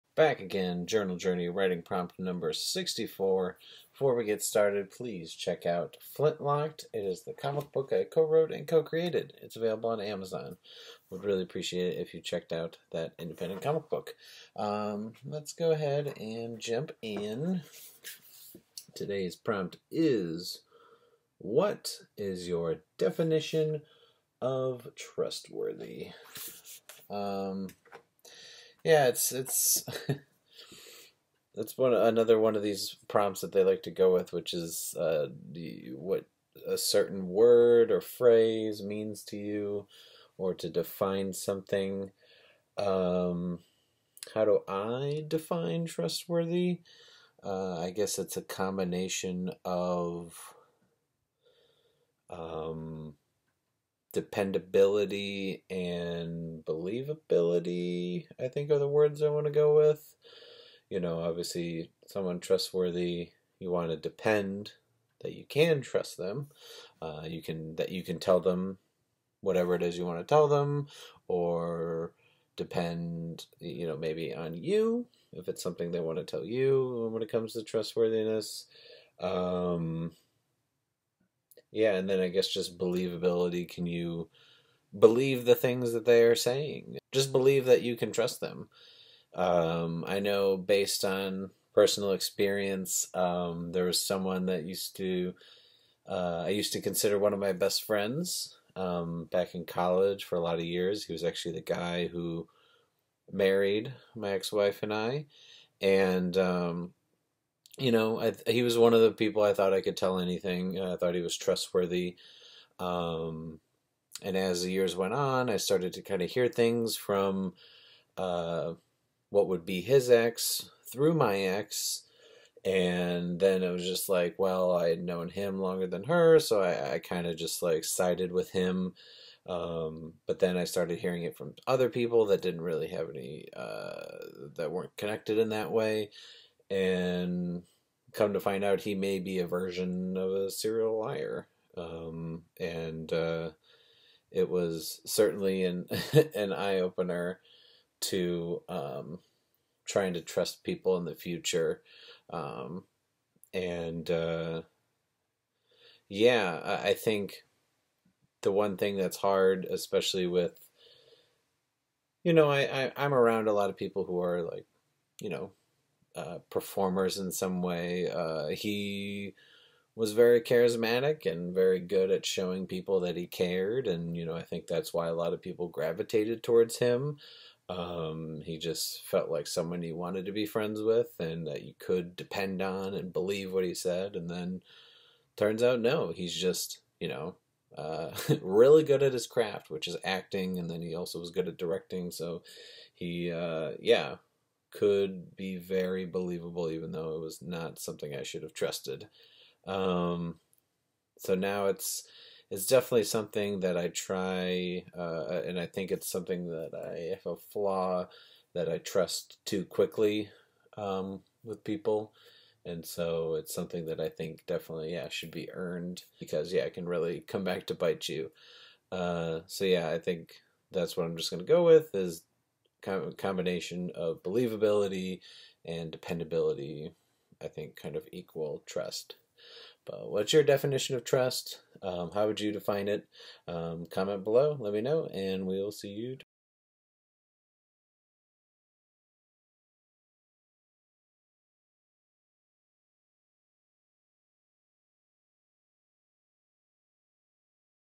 Define Trustworthy? Journal Journey
"300 More Writing Prompts" is a notebook with... it's in the title. I'm going to push myself do a journal and video entry for the next 300 days.
Day 64 prompt: "What is your definition of trustworthy?"

@BrandonProsek on social medias
10/10/2020

My original pirate comic book I co-created:

Wanna see some of our t-shirts?